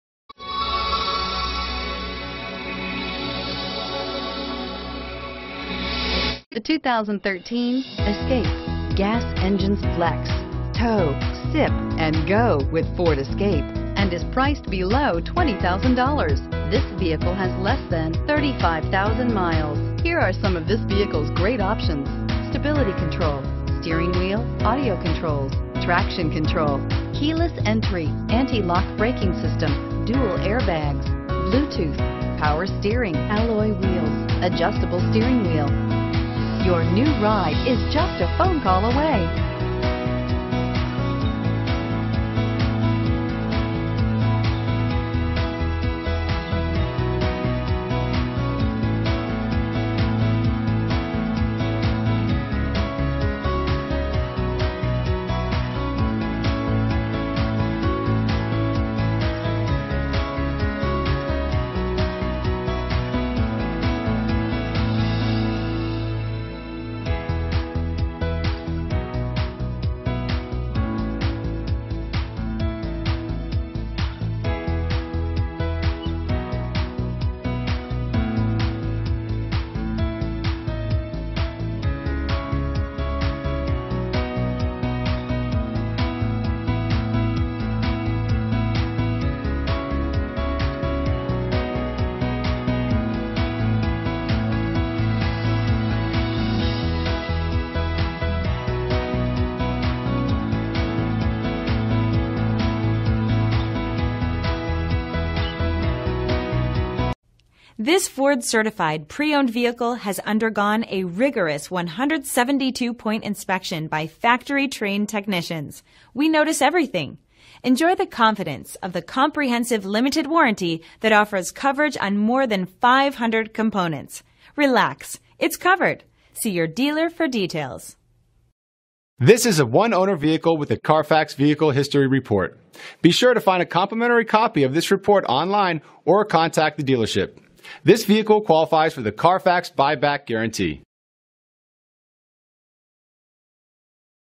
2013 Ford Escape Daytona Beach FL FG1519A
2013 Ford Escape SE

GaryYeomans Ford
1420 North Tomoka Farms Road

Energy-efficient and gas-sipping, this 2013 Ford Escape SE is powered by a fuel efficient Turbocharged Gas I4 1.6L/97 engine that actually saves your hard-earned money. Its Automatic transmission boasts 33 highway mpg and 23 city mpg! It has the following options: Fog Lamps, Wheel nut wrench & jack, Variable intermittent windshield wipers, Urethane tilt/telescoping steering wheel w/speed & audio controls, Traction control, Tire pressure monitoring system (TPMS), SYNC voice-activated communications & entertainment system -inc: Bluetooth capability, steering wheel audio controls, USB port, audio input jack, 911 Assist, vehicle health reports *Additional SYNC services available w/annual subscription fee*, Smart occupant sensing airbags, SIRIUS satellite radio w/6-month prepaid subscription *N/A in AK or HI*, and SecuriLock passive anti-theft system (PATS). Come in for a quick visit at Gary Yeomans Ford Commercial Business, 1420 N Tomoka Road, Daytona Beach, FL 32124 to claim your Ford Escape!